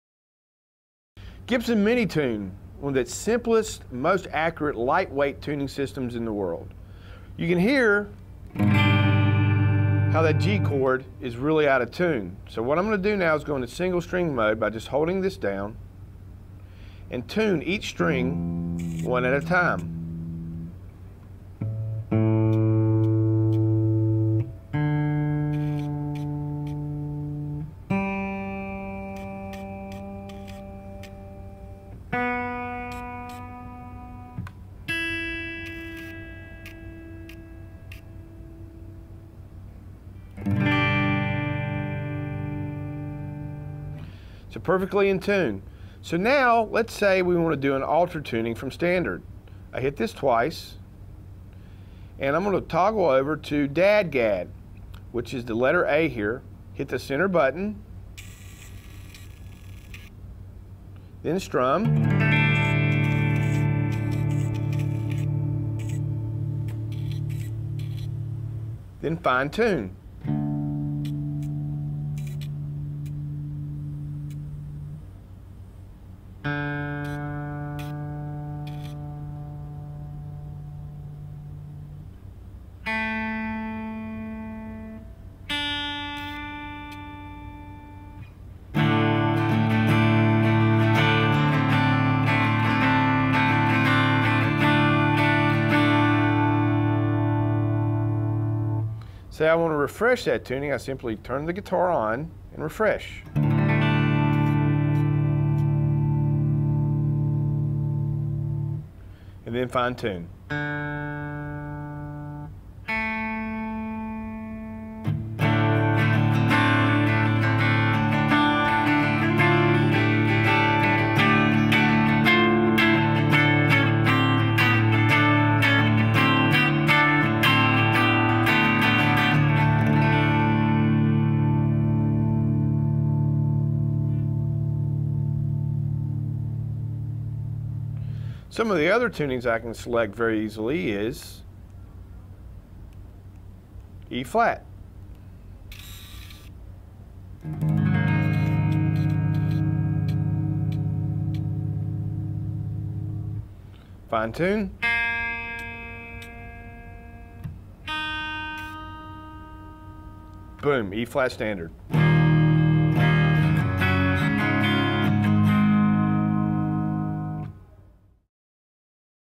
Technology and Science: Gibson Min-ETune 2014 Demo for Pre-Intermediate
Explore the innovative Gibson Min-ETune technology in this 2014 demo tailored for pre-intermediate users. Discover how this groundbreaking tuning system integrates advanced science and technology to revolutionize the guitar-playing experience. Perfect for musicians, tech enthusiasts, and educators looking to understand the fusion of music and modern technology.